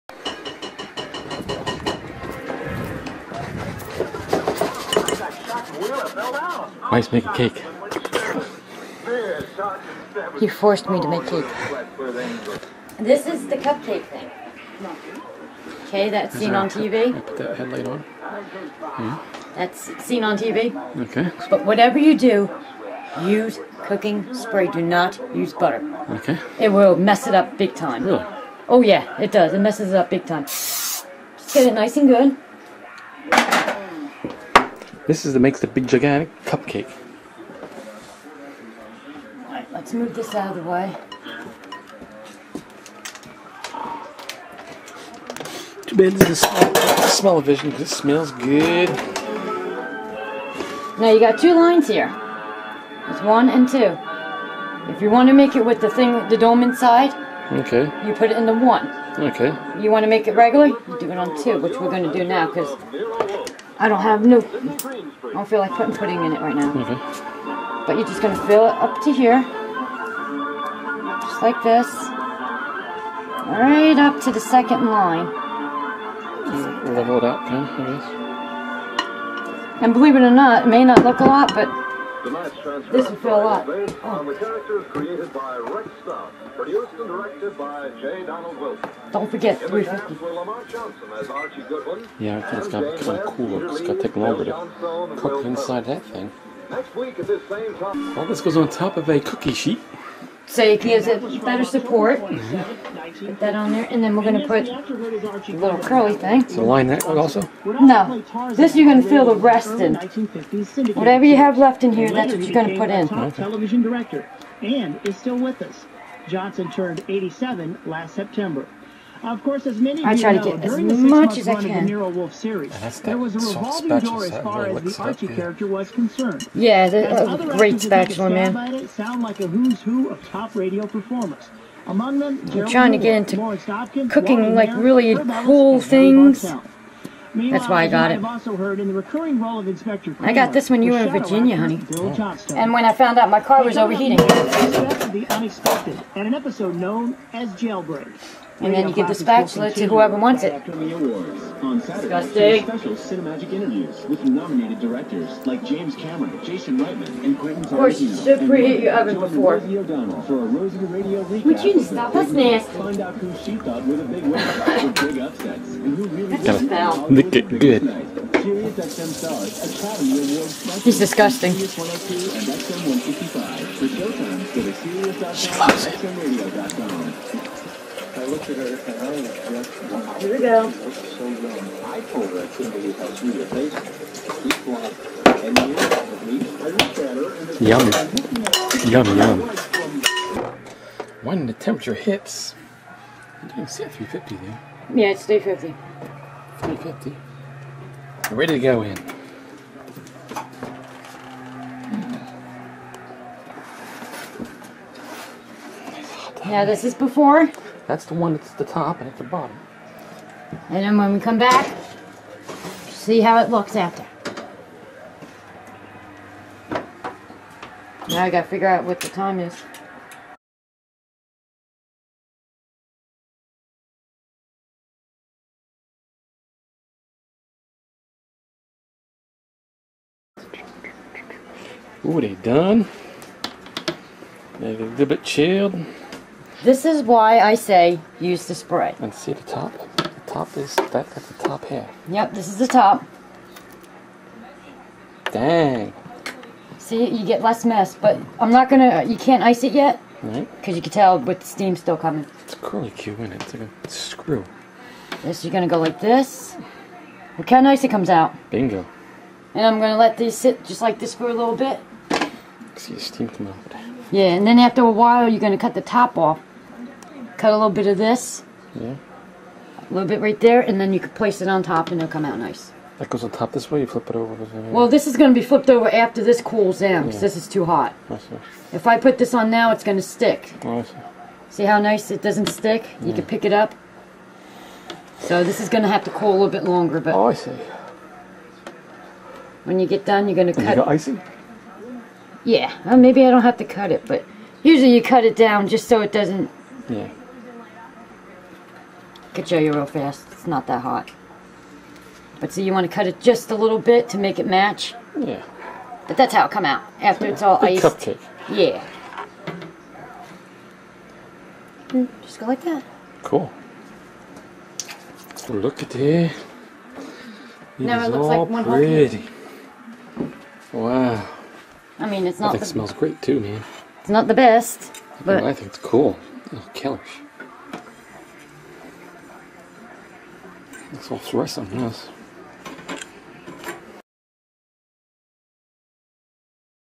Carrot Cupcake
Dawn makes a Carrot Cupcake in those BIG cupcake preforms.